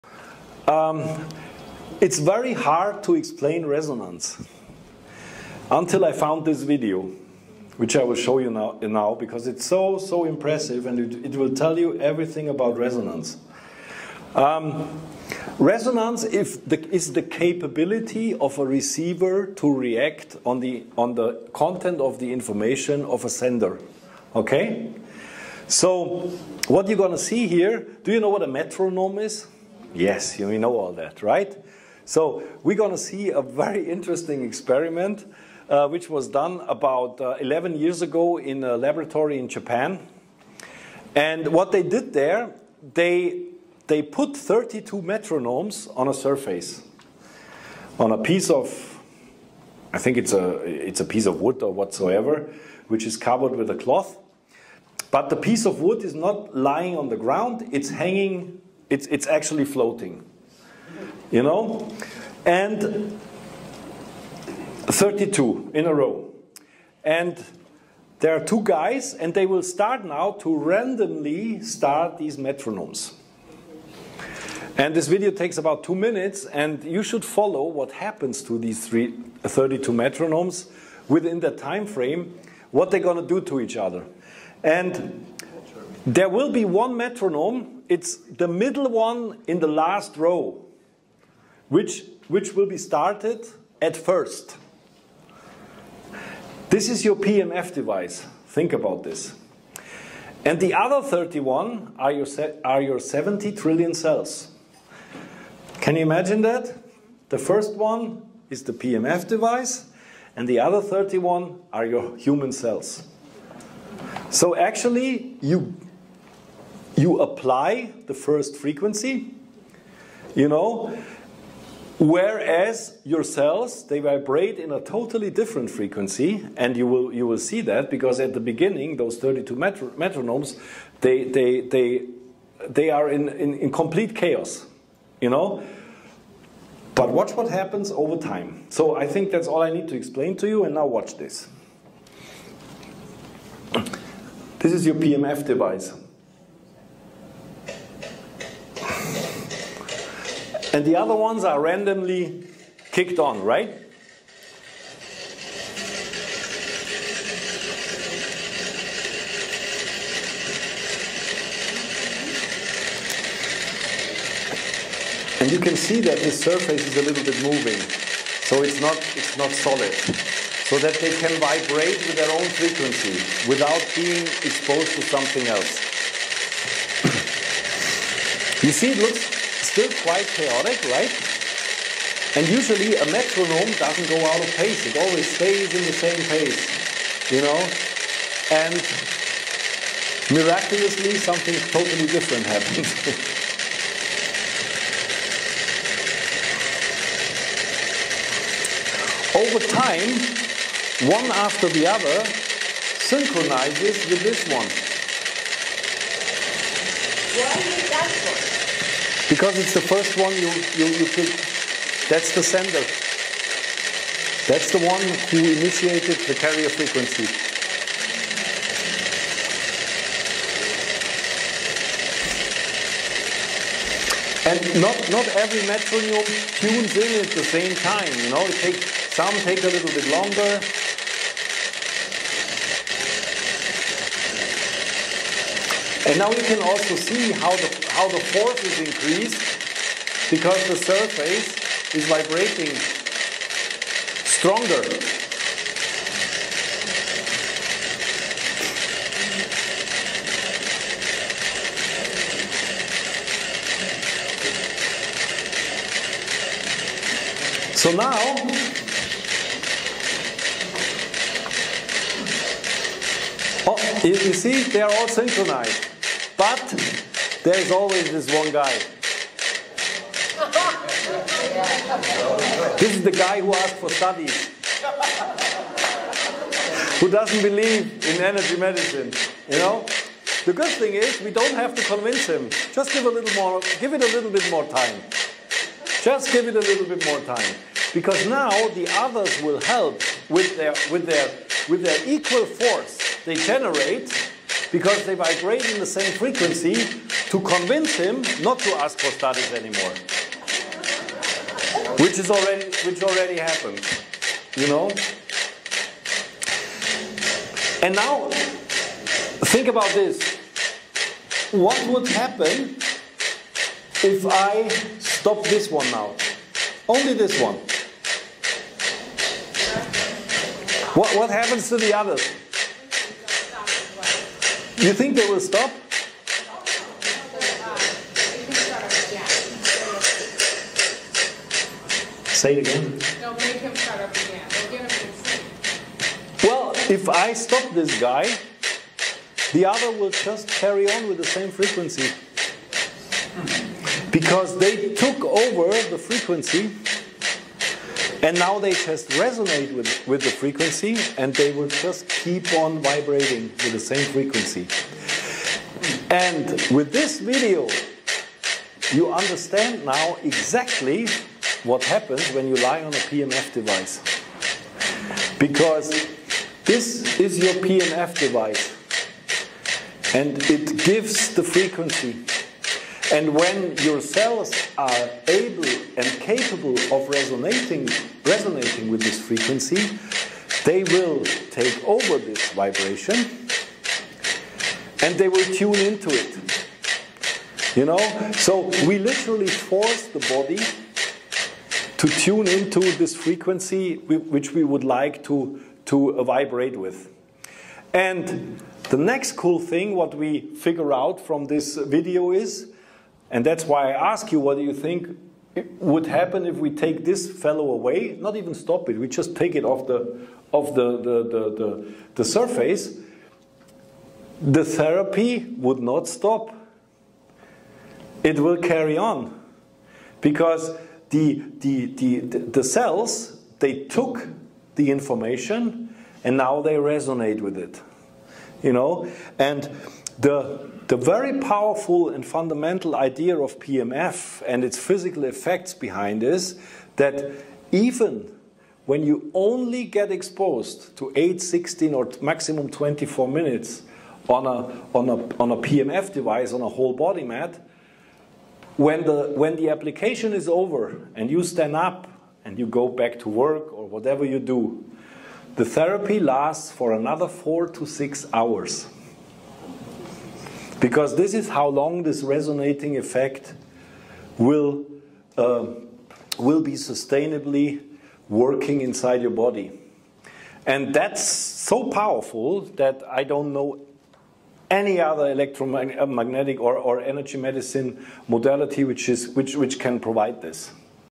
PEMF RESONANCE
Wolfgang explains resonance (PEMF) at Wolfgang Event, May 13, 2023 at CH7 of Laguna Woods Village, S. CA. by Terry Willis